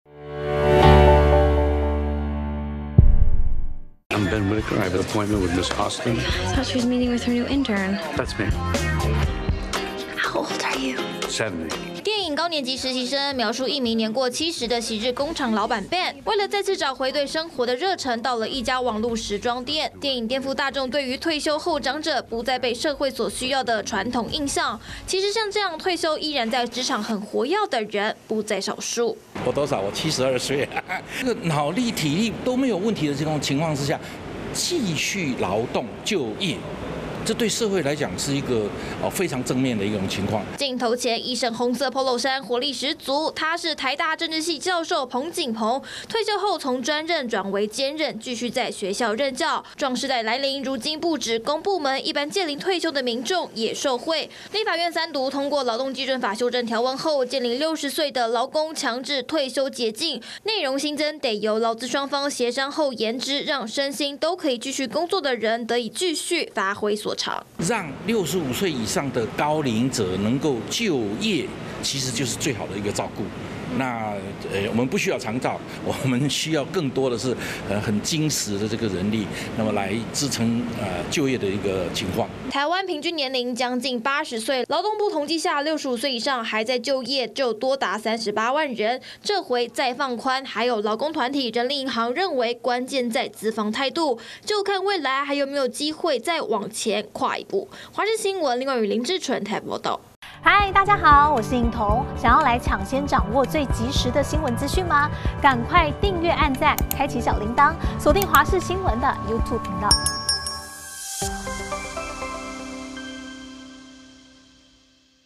勞工65歲強制退休解禁！ 可經勞雇雙方協商延後｜華視新聞 20240715
為了因應老人化社會及壯世代，立法院今(15)日三讀通過《勞動基準法修正條文》，放寬勞工年滿65歲強制退休的規定，只要雇主經勞雇雙方協商後，得延後強制勞工退休年齡。像是現年72歲的台大政治系教授彭錦鵬就說，自己退休後就是從專任轉為兼任教授，繼續在學校任傳授學生知識。